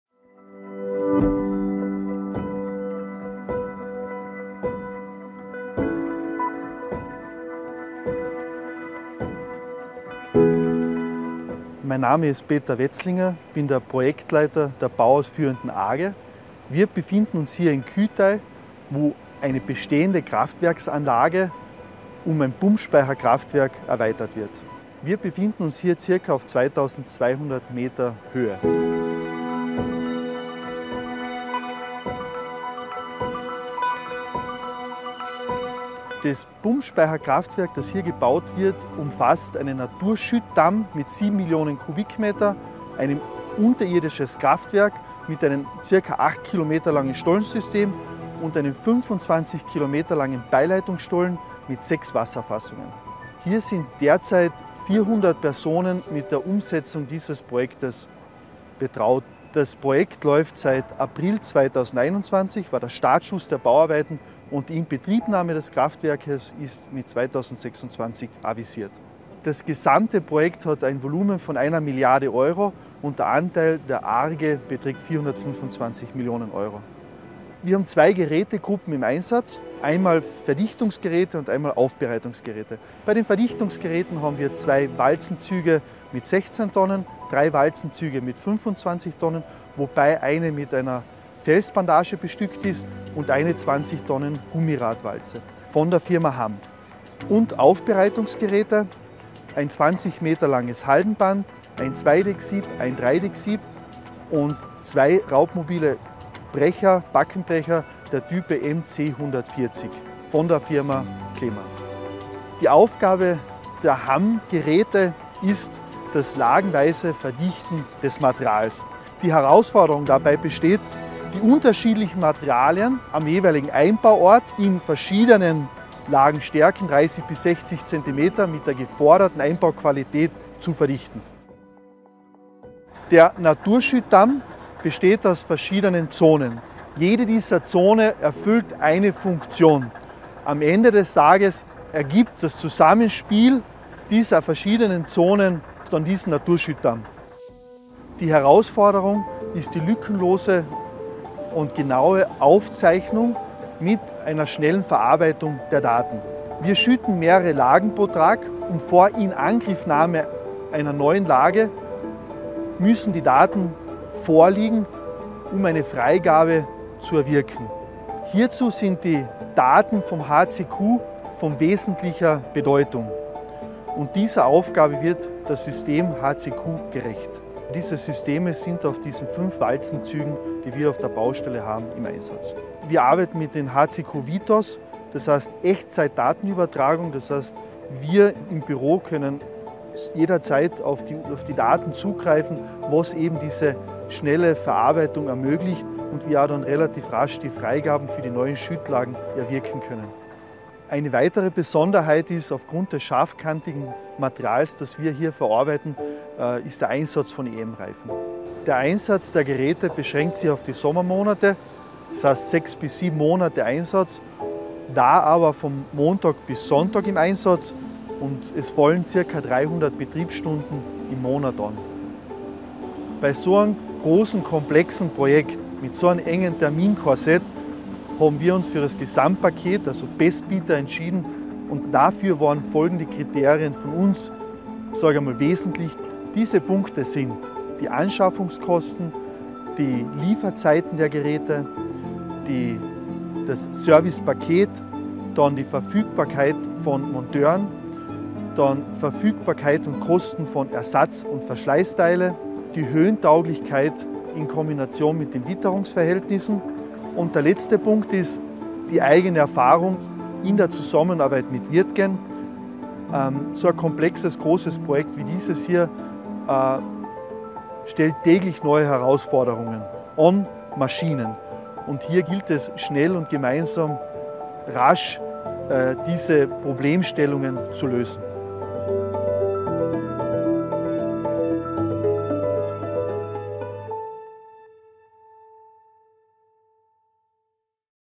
Großprojekt mit Hamm und Kleemann auf über 2.100 m Höhe
Oberhalb des Tiroler Bergdorfs Kühtai entsteht derzeit ein Wasserkraftwerk zur Erzeugung von Strom. Der künftige Speichersee wird ca. 31 Mio. Kubikmeter Wasser stauen. Dafür entsteht seit April 2022 ein 130 m hoher Damm aus Naturstein mit einem Schüttvolumen von 6,9 Mio. Kubikmetern. Schnelligkeit, Qualität und Effizienz sind hierbei tagtäglich gefordert – ein Job für die Maschinen der Wirtgen Group. Mehr zu diesem komplexen Bauprojekt im Video mit Bildern aus dem September 2022.